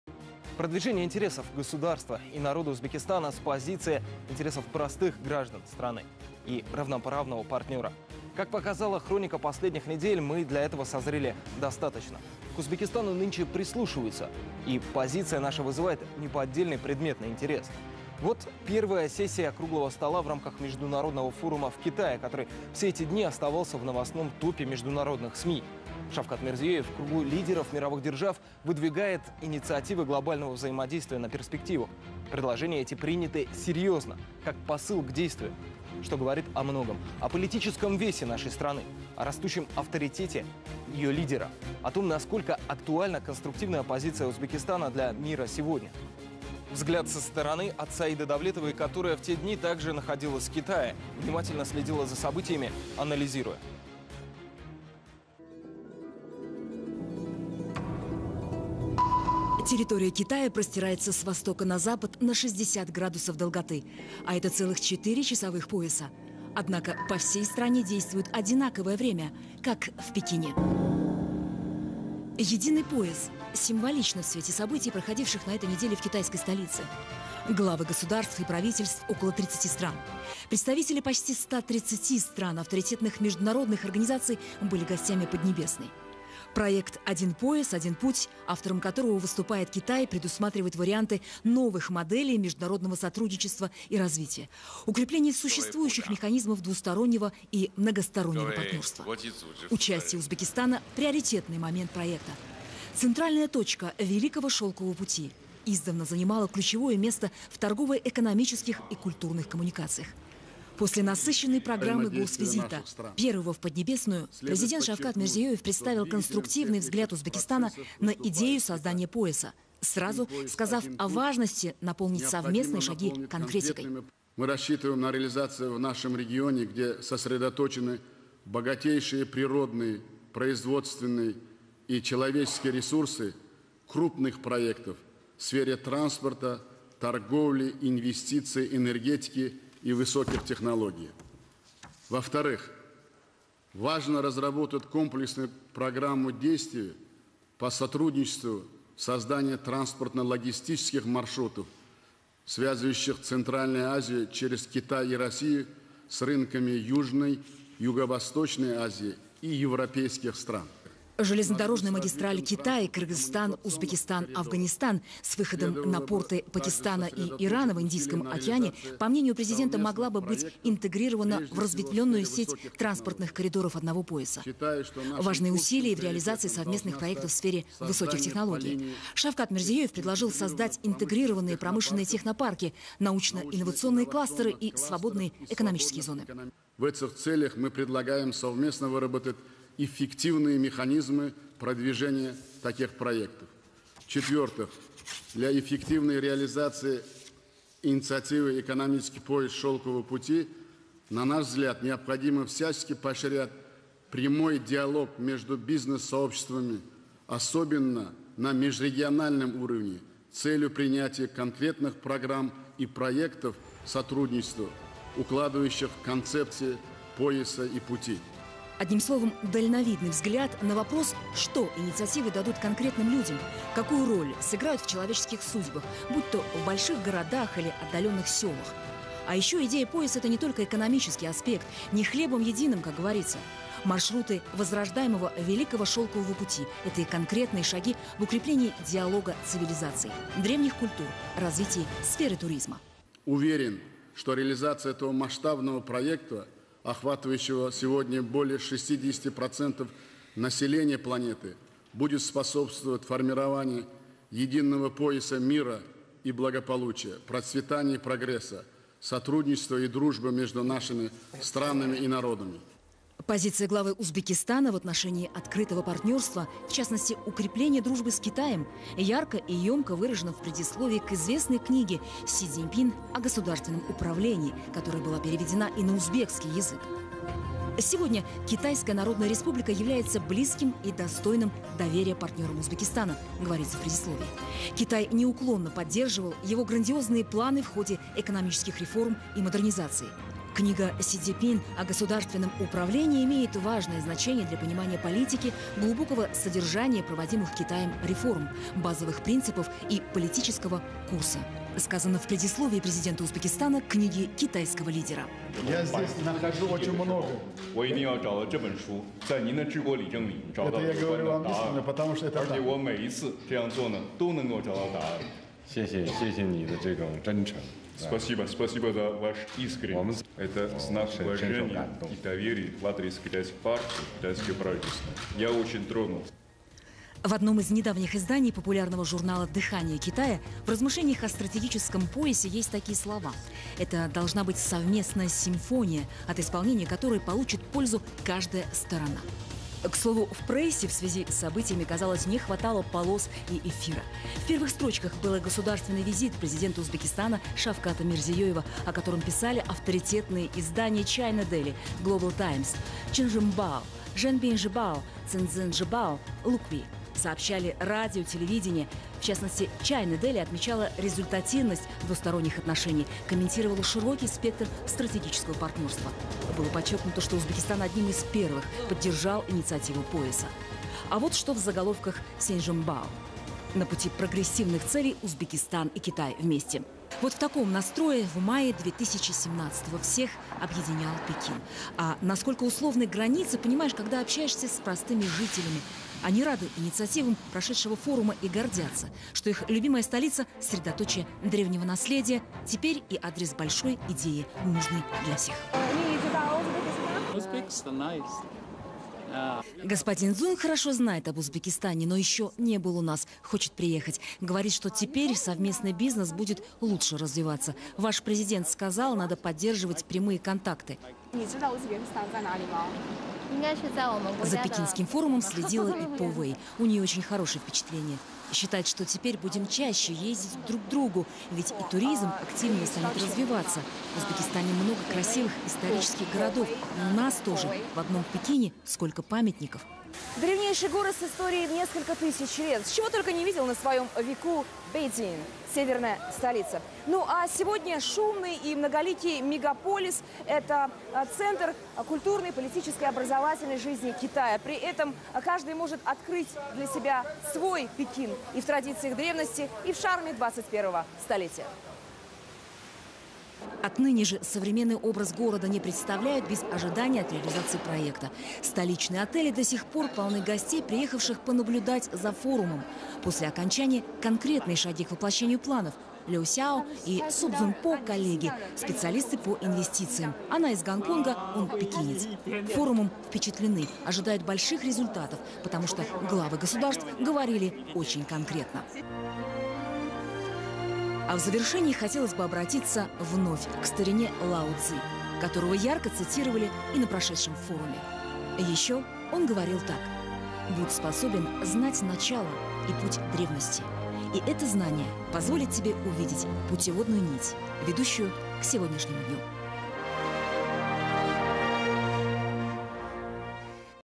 Президент Узбекистана принял участие в форуме "Один пояс, один путь"
В Пекине начался форум «Один пояс, один путь»

Президент Республики Узбекистан Шавкат Мирзиёев 14-15 мая в Пекине принял участие в международном форуме "Один пояс, один путь".